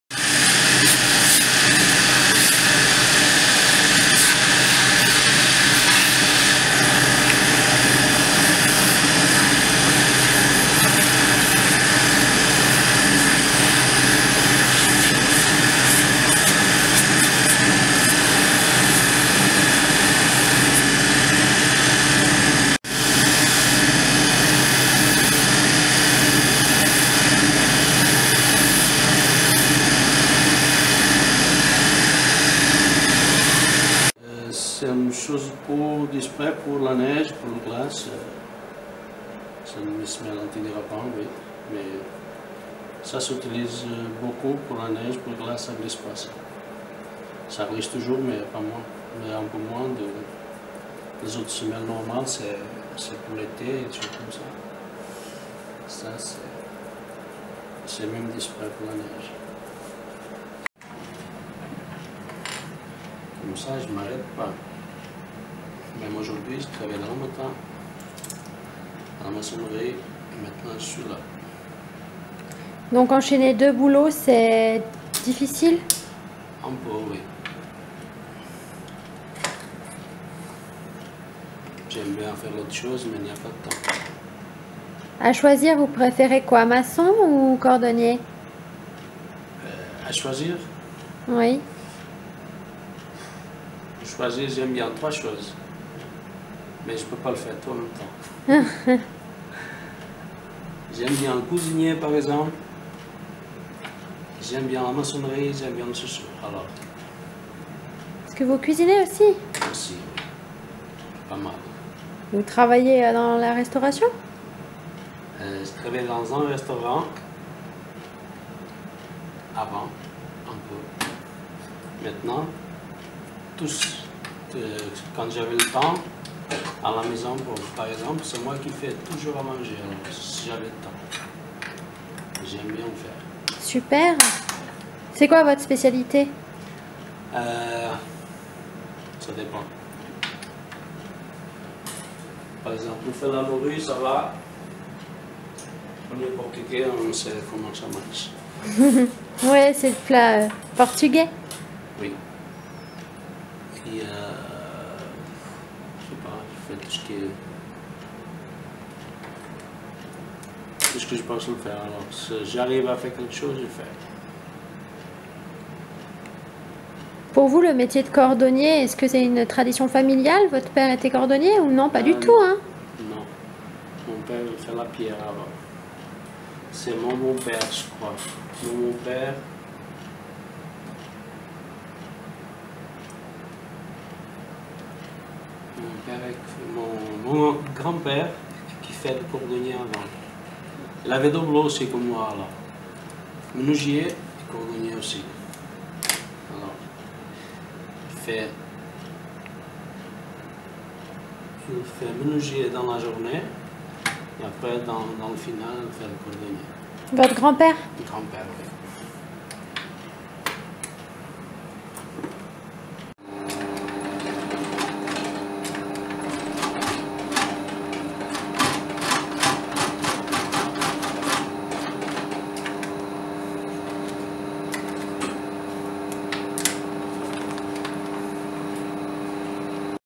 film cordonnier
Cordonnier est un métier ancestral. A Bourg-Saint-Maurice, en haute-tarentaise, Filip Lopez a ouvert son atelier il y a trois. En parallèle, son activité principale est maçon. C'est le soir et les week-end, que Filip Lopes assure la réparation des chaussures et le travail du cuir. Plongé dans l'art de la cordonnerie à l'âge de 13 ans, il satisfait de nombreuses demandes dans la vallée tarine. Passionné, il nous a ouvert les portes de sa boutique, et de son savoir-faire.